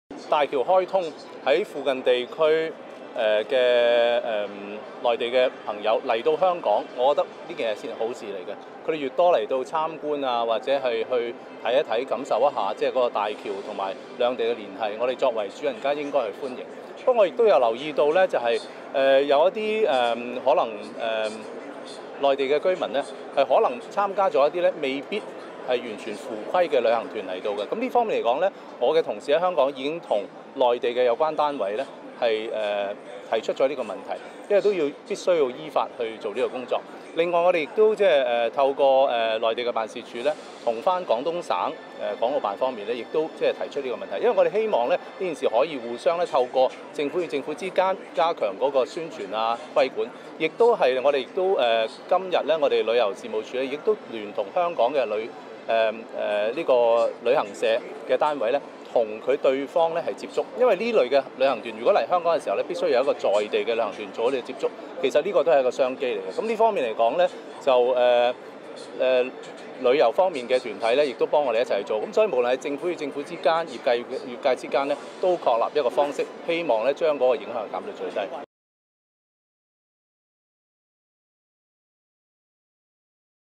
政府表示會跟廣東省商討 冀源頭處理非法團無秩序來港
20181106

港珠澳通車後第二個周日（11月4日），利用港珠澳大橋出入境香港人次首破10萬，全日有102,919人次，東涌被旅客「迫爆」。運輸及房屋局長陳帆昨日表示，當局已調動穿梭巴士約2000班次，認為整體交通和人流相對有秩序和暢順。到了昨日（周一）， 大橋人數即告回落，昨日至晚上8時暫有5.58萬人次出入境。